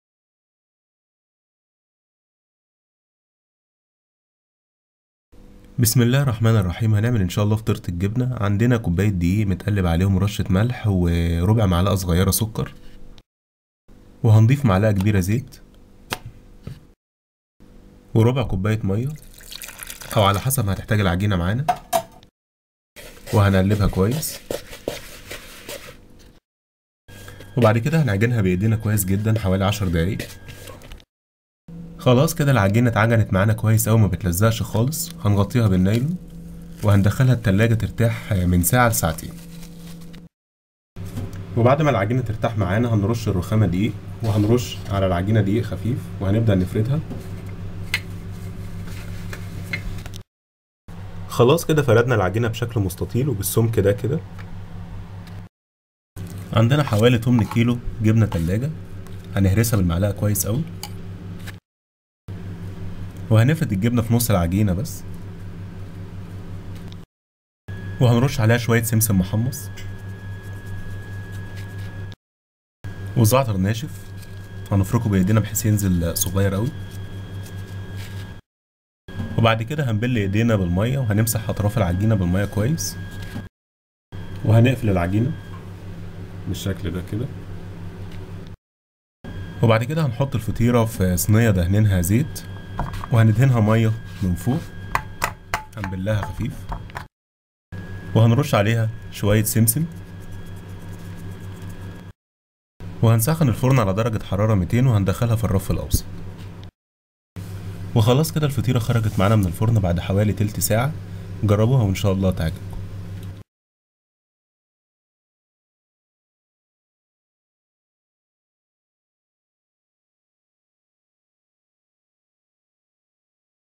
طريقة عمل فطيرة الجبنة بالزعتر و السمسم - Cheese Pie
طريقتنا الخاصة لعمل أفضل فطيرة فطاطري بالجبنة و الزعتر و السمسم في الفرن - وصفة خاصة من سنة أولى مطبخ .
لمشاهدة الوصفة مكتوبة مع المقادير و الطريقة اضغط على الرابط التالي
ـــــــــــــــــــــــــــــــــــــــ
يمكنكم متابعتنا على شبكات التواصل الاجتماعى على :